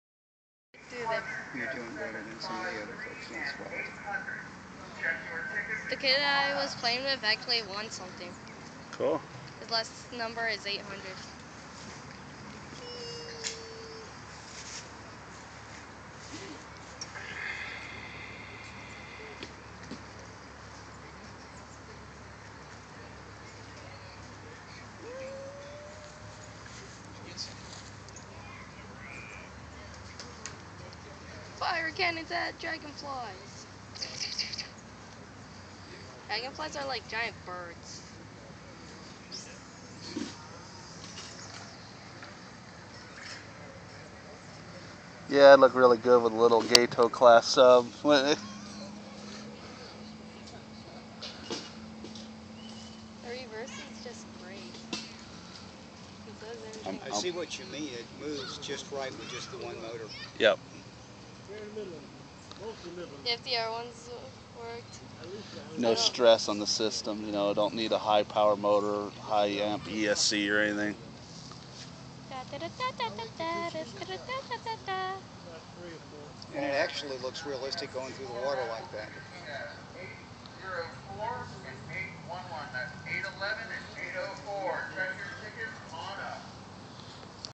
sterling battleship rc model
cruising around in lake seminole park, a smaller pond used for the rc boats.
this is the 2012 BAY AREA ELECTRIC BOATERS annual regatta. MY 1st regatta for scale boats. great guys, i will be back.